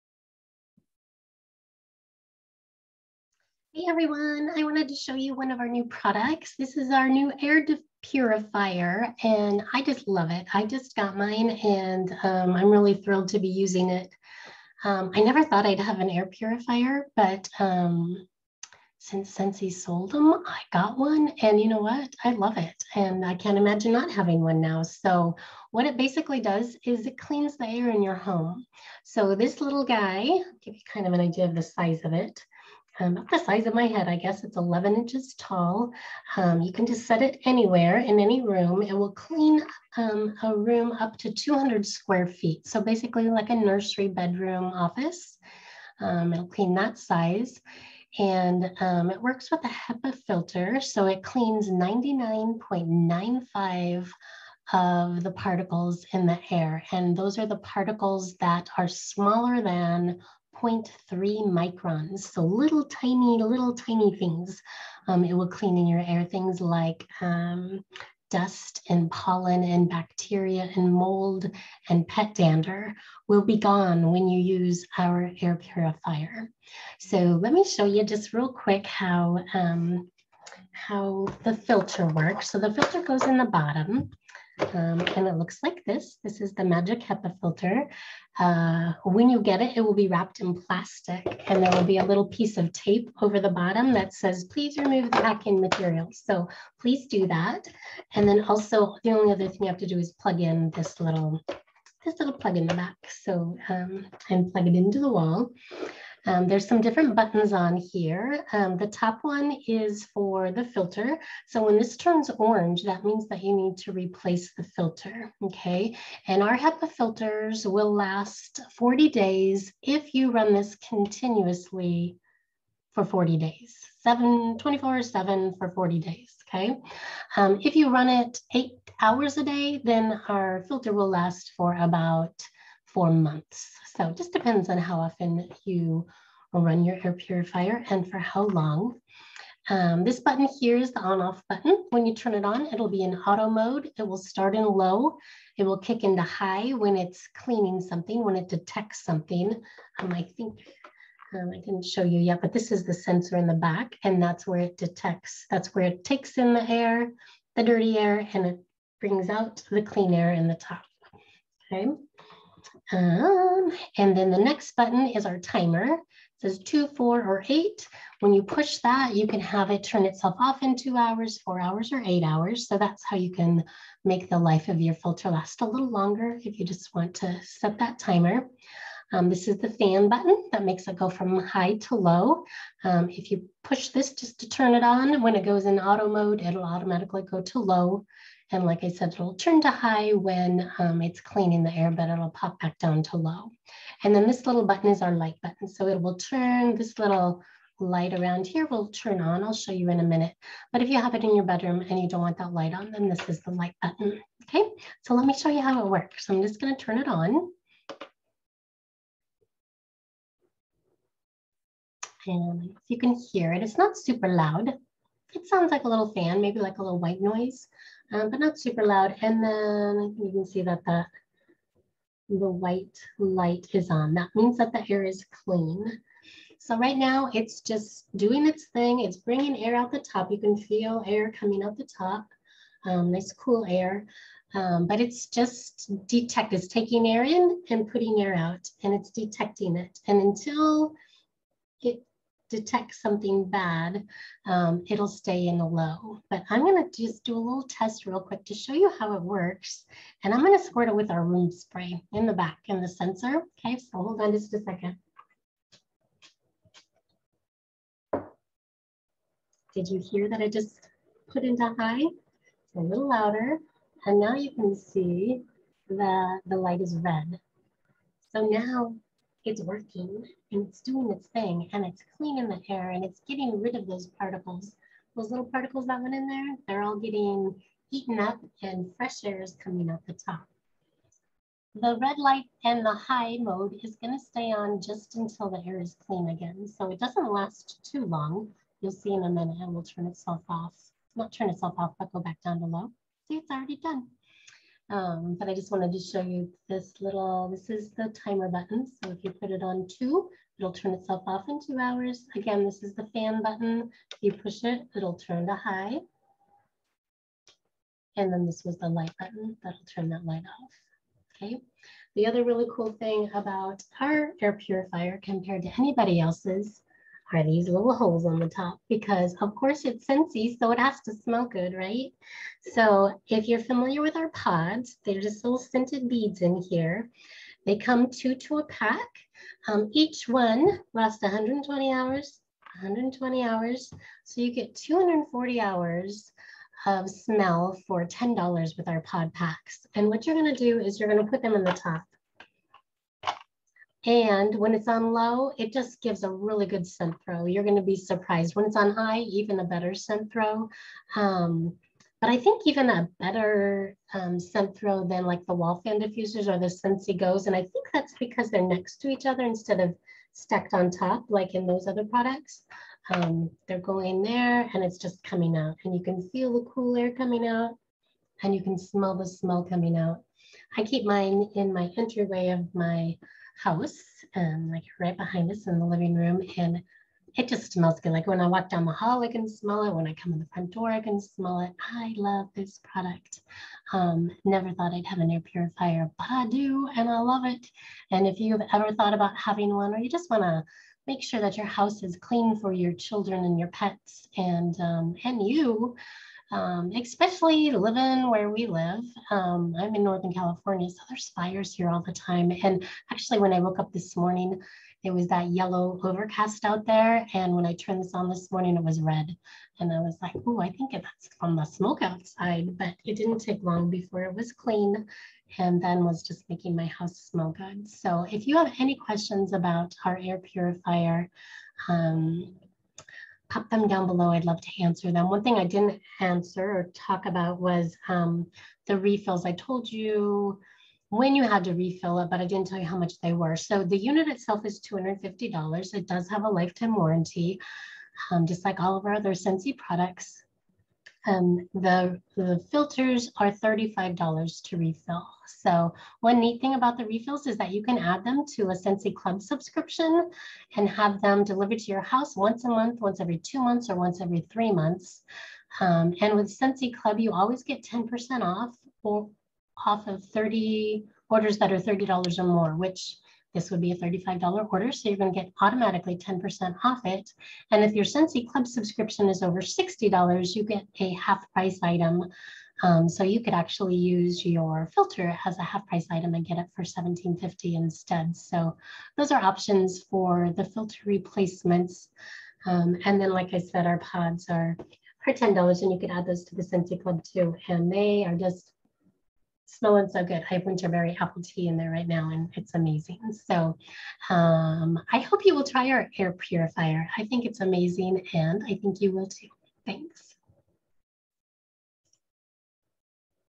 Check Out Scentsy's Air Purifier!
Our Air Purifier not only cleans the air, but it smells good too & comes with a lifetime warranty, just like all of our products.